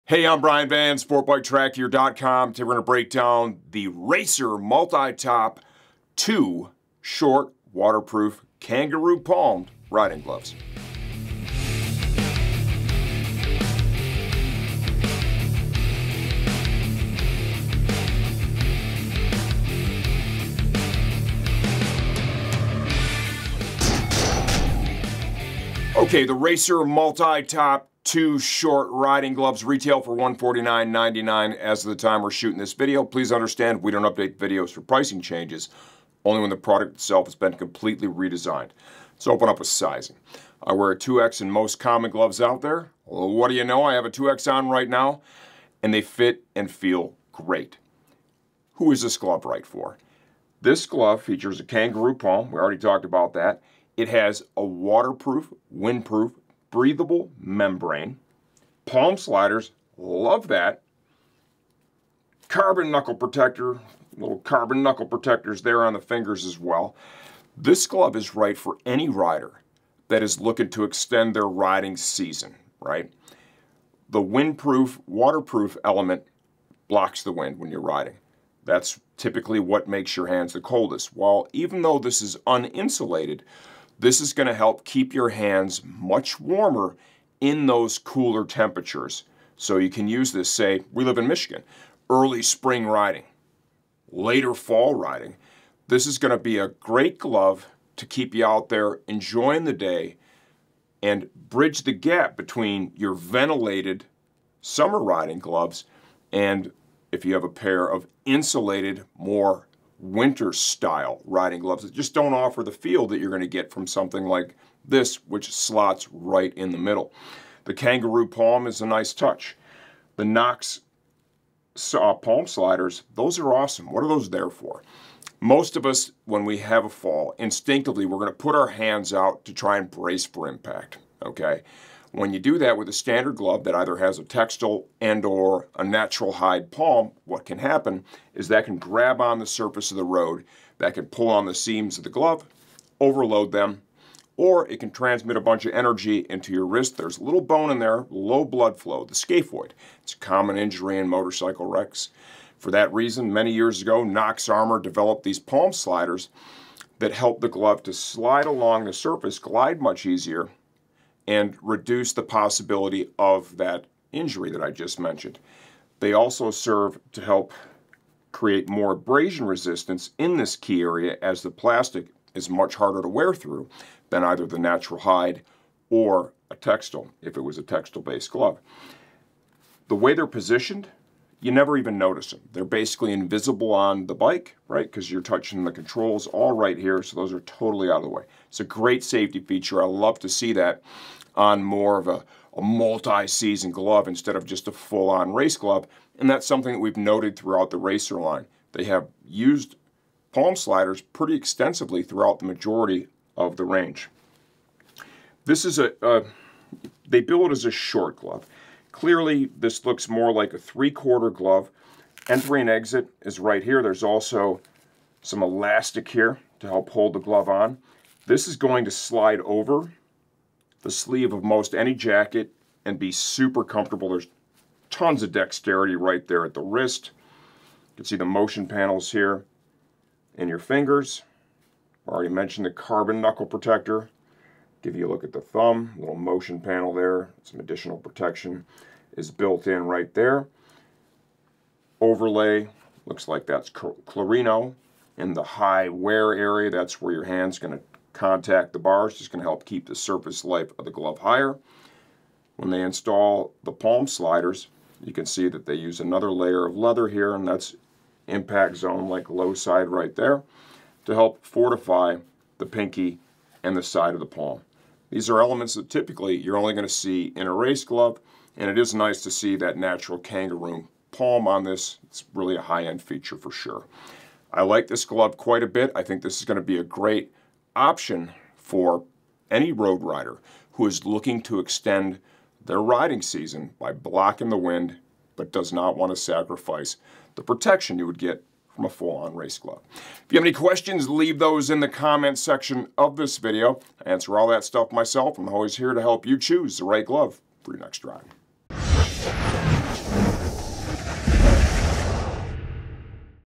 Racer MultiTop Short Waterproof Motorcycle Gloves Review from SportbikeTrackGear.com
In this video Van breaks down the Racer MultiTop Short Waterproof Gloves. These are an excellent waterproof short riding glove that will work great for any rider looking for a comfortable waterproof glove that delivers race level protection.